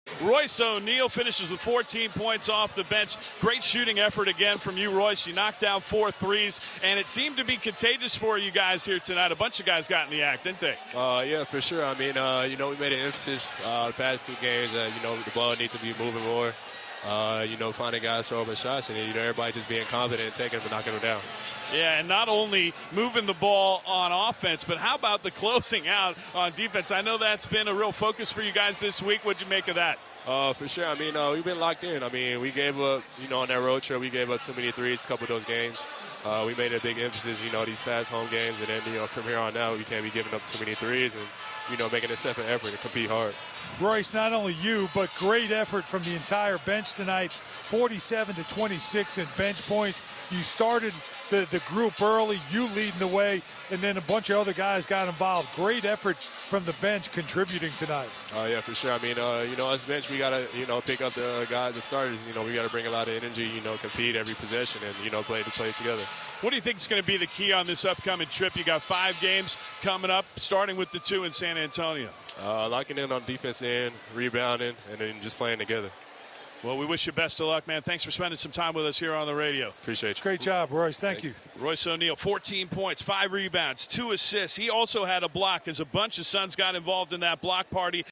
Royce O'Neale says Suns are shooting with confidence after back-to-back wins
Video: Felisa Cárdenas and Jeremy Schnell/Arizona Sports